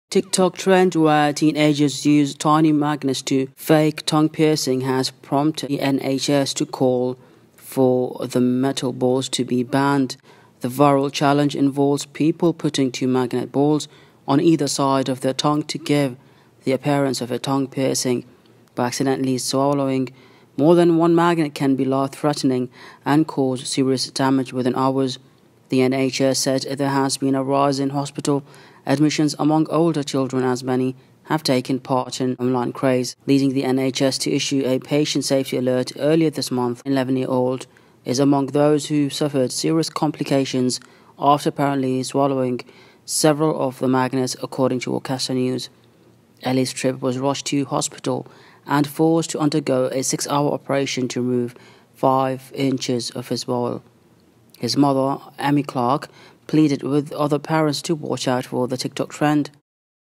Tiktok challenge | tongue piercing
A TikTok trend where teenagers use tiny magnets as fake tongue piercings has prompted the NHS to call for the metal balls to be banned.
The viral challenge involves people putting two magnetic balls on either side of their tongue to give the appearance of a tongue piercing.

But accidentally swallowing more than one magnet can be life-threatening and cause serious damage within hours.
The NHS said there has been a rise in hospital admissions among older children as many have taken part in the online craze, leading the NHS to issue a patient safety alert earlier this month.
An 11-year-old is among those who suffered serious complications after apparently swallowing several of the magnets, according to Worcester News.

Ellis Tripp was rushed to hospital and forced to undergo a six-hour operation to remove five inches of his bowel.

His mother, Amy Clarke, pleaded with other parents to watch out for the TikTok trend.